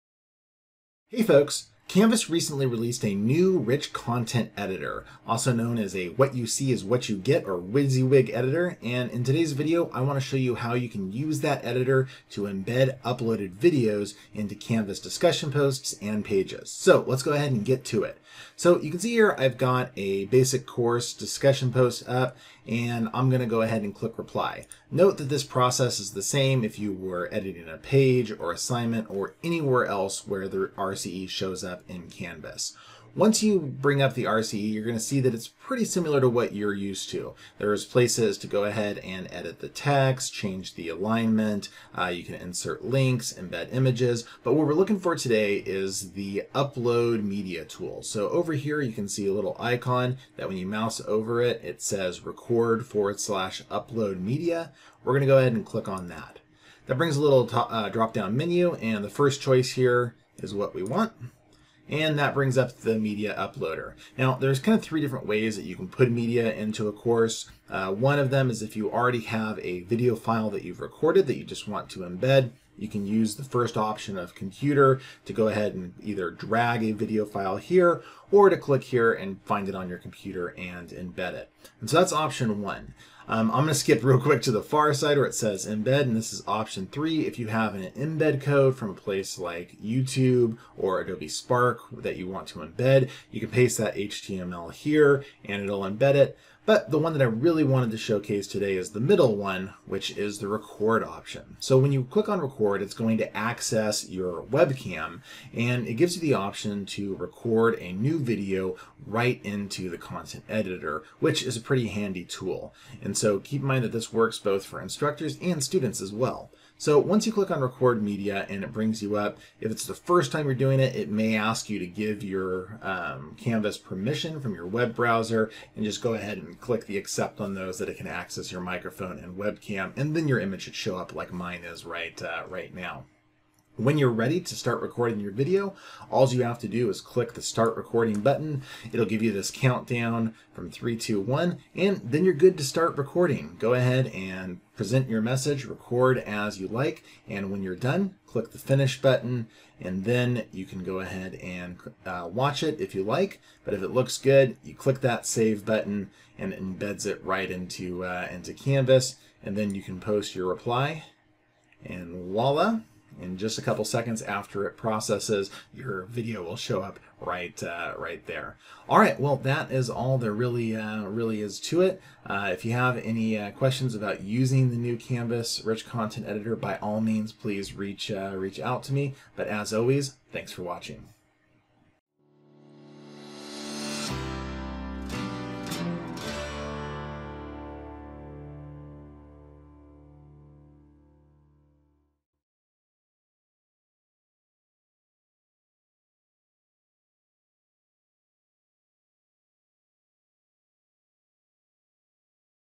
How to Record and Post Videos in Canvas with the New Rich Content Editor (Remote Instruction Series)
This video shows how you can record and embed video into Canvas using the new Rich Content Editor that was launched mid 2020. This series is aimed at helping folks figure out easy methods of adapting materials from face-to-face classes to online modalities during the Covid-19 pandemic.